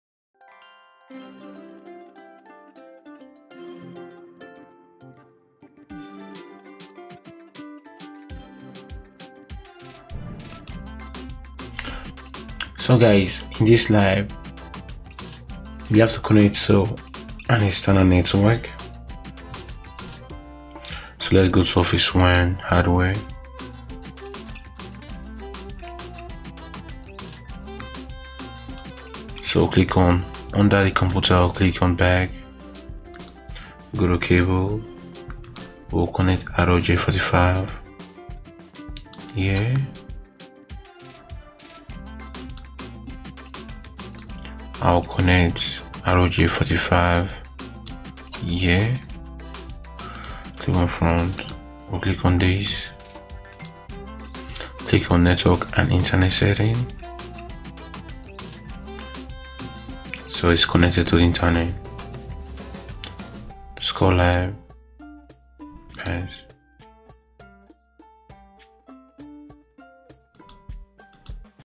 TestOut LabSim: Connect to an Ethernet Network
You are a network technician for a small corporate network. The owner of the company has asked you to connect a new workstation to the network while the new employee is in an orientation meeting.

In this lab, your task is to:

Connect the workstation in Office 1 to the Ethernet local area network using a twisted-pair cable.
Confirm that the Office 1 workstation is connected to the local network and the internet.